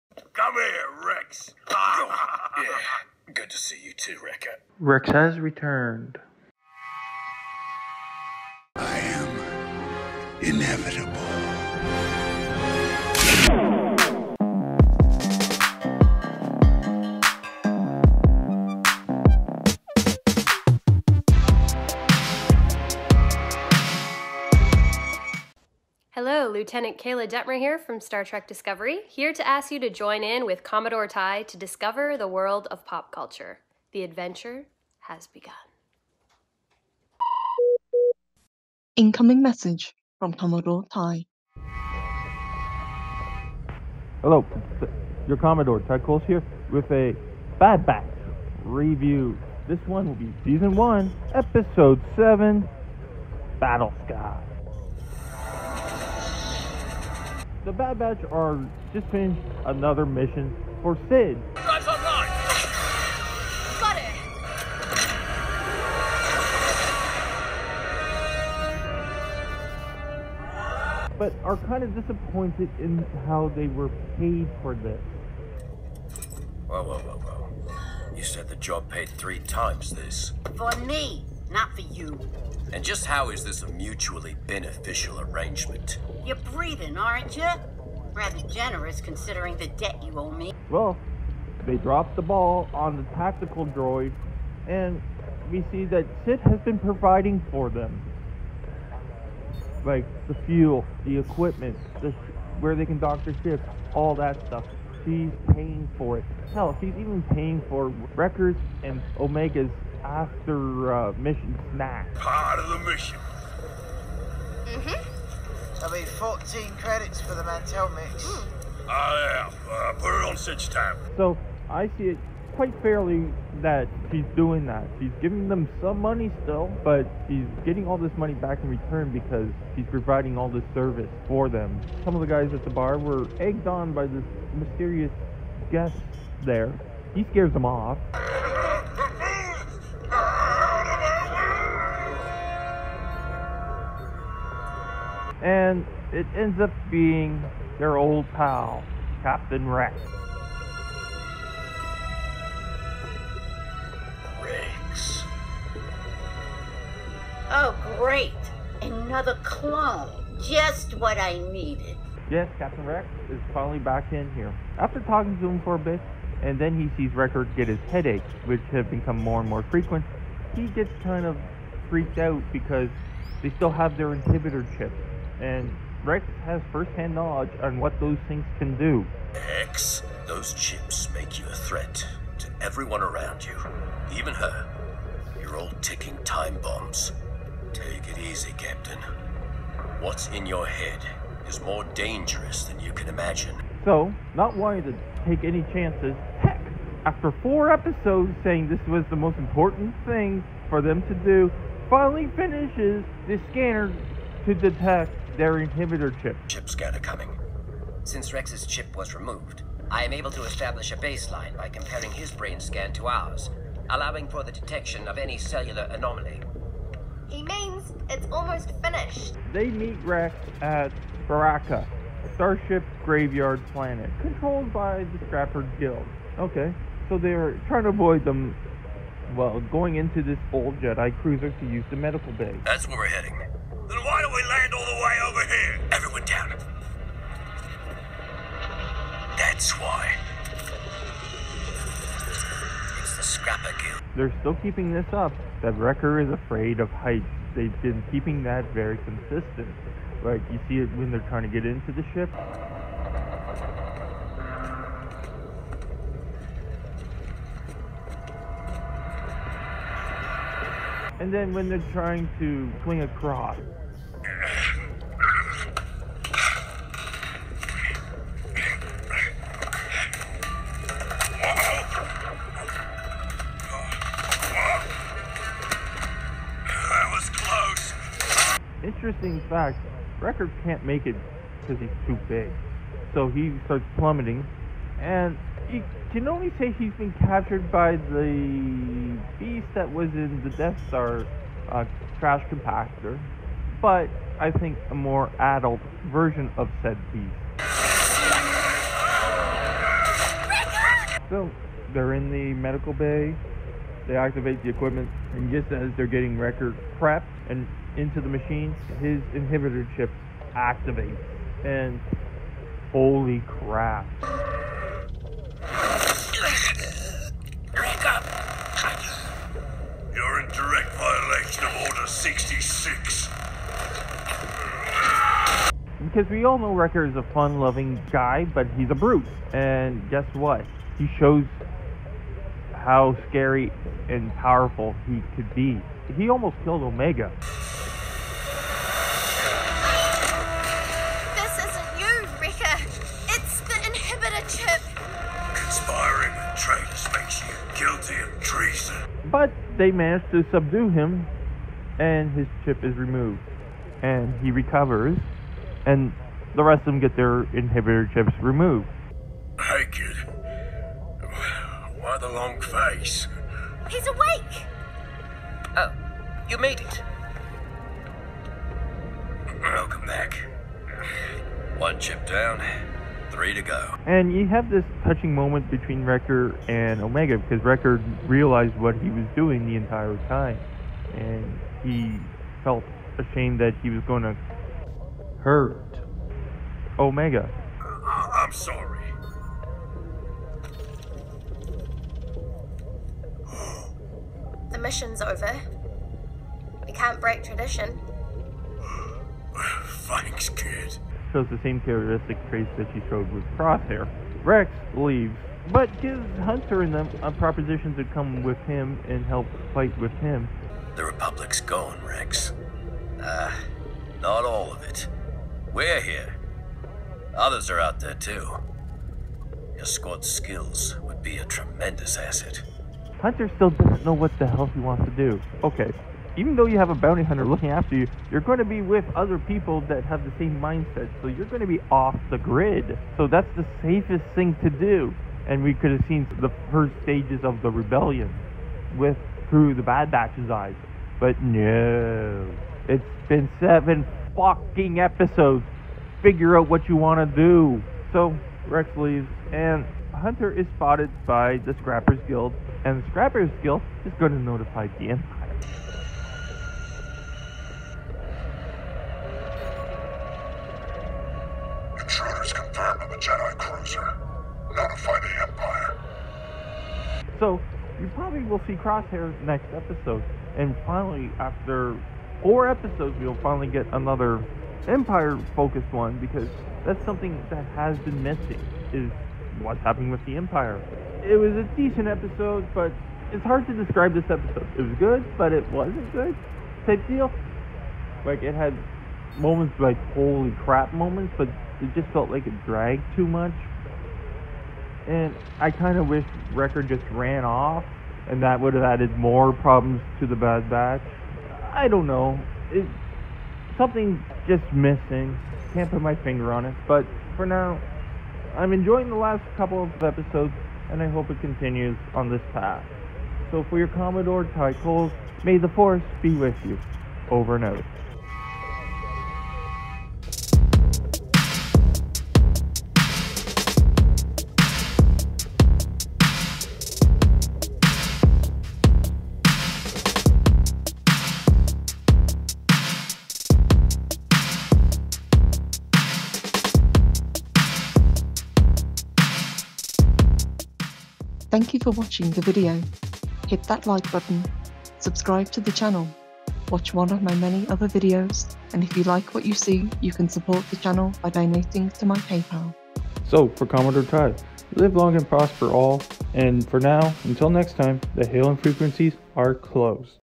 Star Wars: The Bad Batch REVIEW - S.1 Ep. 7 "Battle Scars"
After their failure to return with the tactical droid's data, Cid tells the Bad Batch that they will need to do a big job to pay off their debt to her. They are interrupted by Trace and Rafa's mystery client, their old friend Captain Rex, who is alarmed to learn that the Bad Batch have yet to remove their inhibitor chips like he has. They go to Bracca, a starship graveyard planet controlled by the Scrapper Guild, and sneak onto an old Jedi cruiser to use its medical bay. Wrecker's agitation activates his chip and he attacks his teammates, but he is subdued and the chip removed. Rex takes his leave once all the chips are gone; as they say goodbye, Hunter is seen by members of the Scrapper Guild.